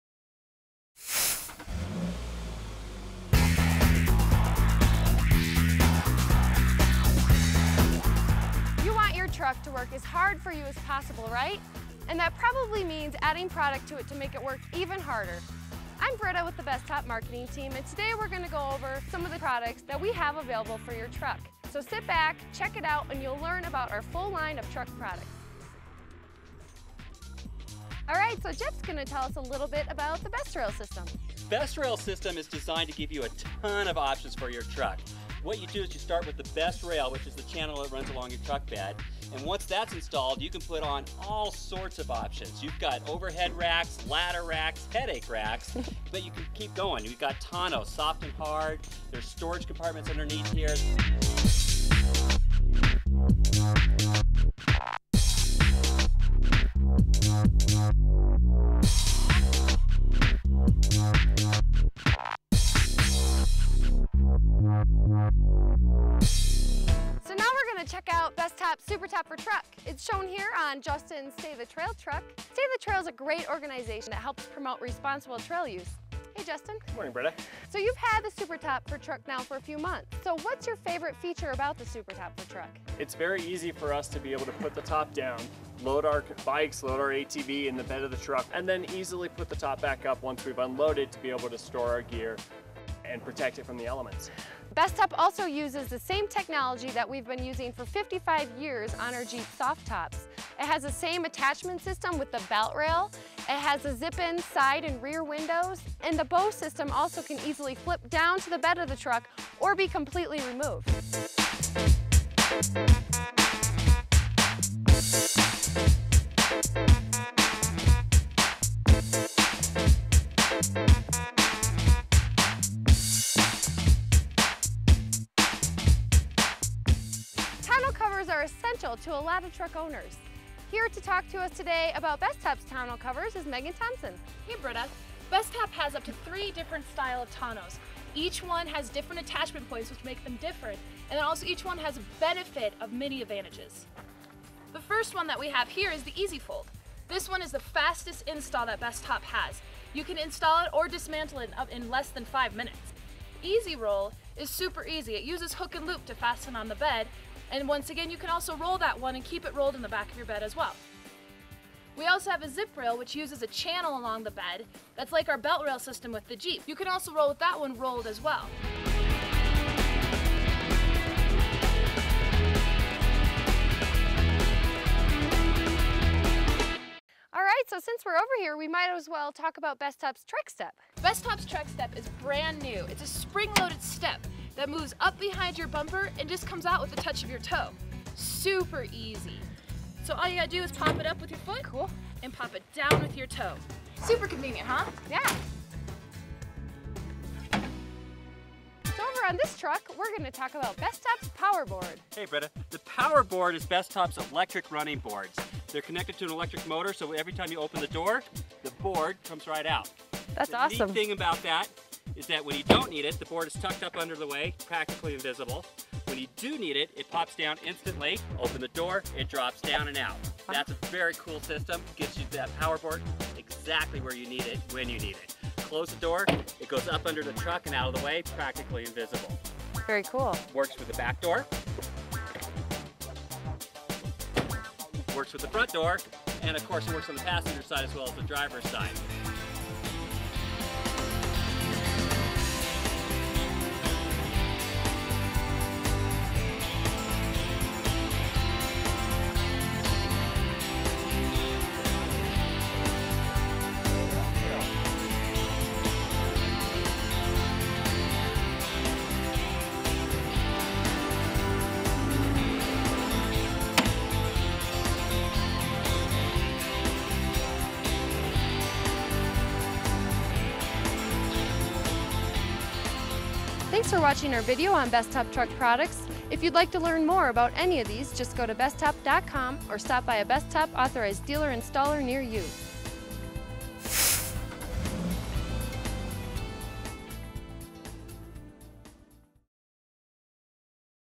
Bestop Truck Products
Bestop produces a variety of products for trucks. Here's a quick overview of BestRail, TrekStep, PowerBoard, and Tonneau Covers (ZipRail, EzRoll and EZFold).